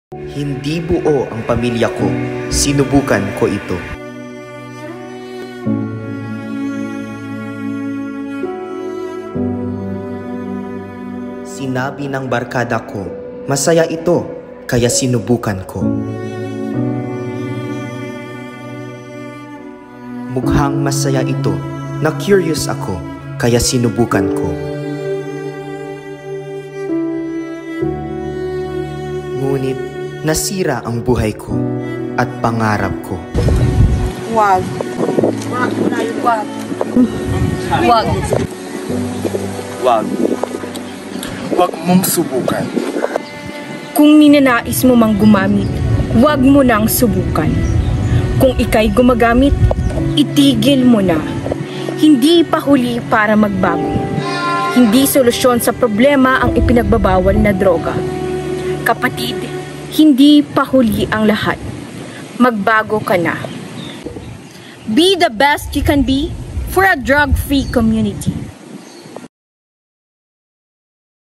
Tiktok Entry No. 3 - "Happy Towards Drug Free Philippines"
A tiktok presentation by Team MingMing with the theme "Happy Towards Drug Free Philippines" during the WEMBOREE 2022 held at Lingayen, Pangasinan.